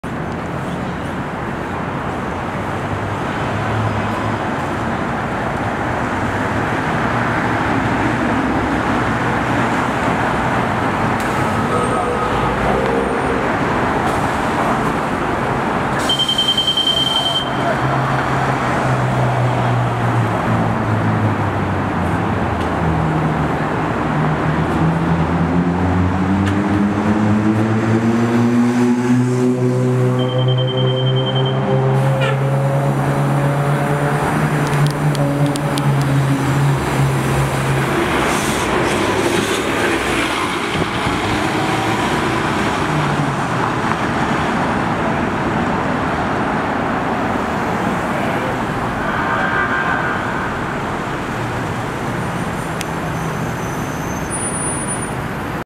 Chicago Transit Authority 2012-2013 New Flyer D60LFR 4377 On The 147
Seen here is New Flyer D60LFR 4377 operating on the 147 to Howard Station making its way up North Michigan Avenue in Downtown Chicago. This video was taken at North Michigan Avenue & East Walton Place. This video was taken in December of 2022.